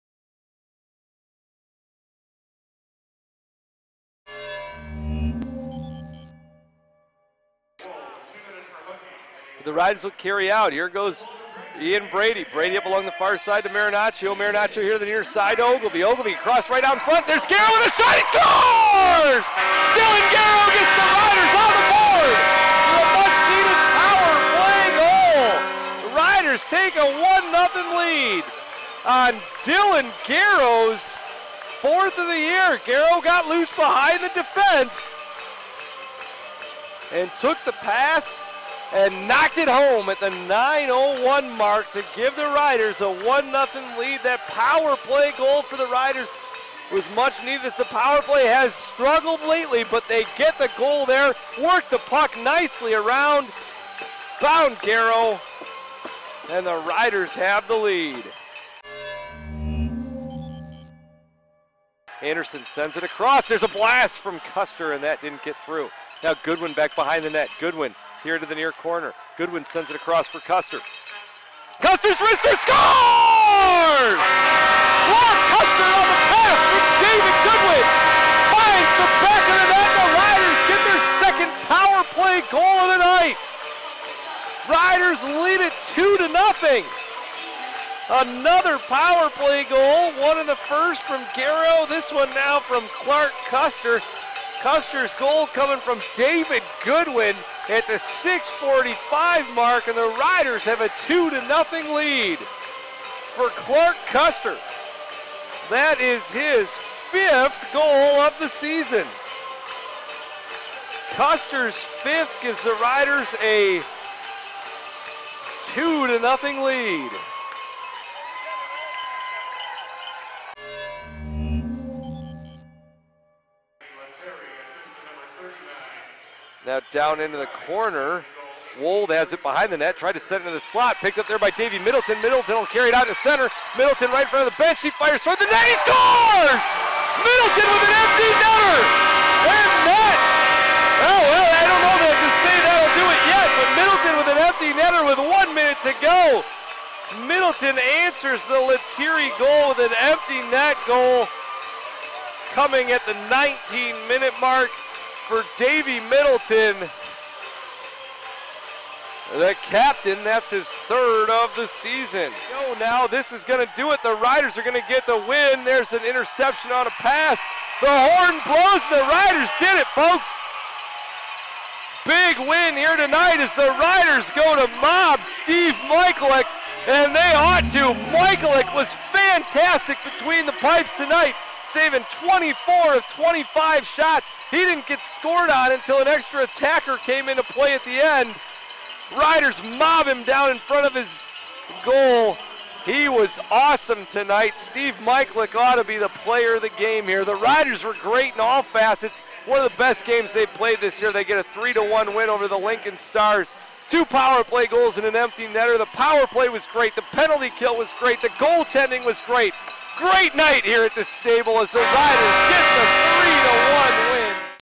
December 12, 2012 Highlights vs. Lincoln
The RoughRiders won 3-1 over Lincoln. Goals by: Dylan Gareau, Clark Kuster and Davey Middleton.